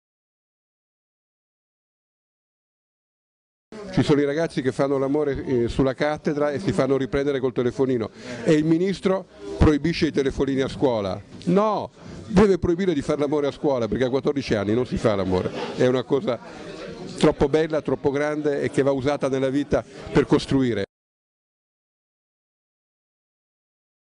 Buttiglione e l'età giusta per fare l'amore
A 14 anni non si fa l'amore, è una cosa troppo bella e troppo grande, che va usata nella vita per costruire". Queste parole rappresentano il Buttiglione-pensiero sul sesso e l'età giusta per praticarlo. Quando si parla di esperti...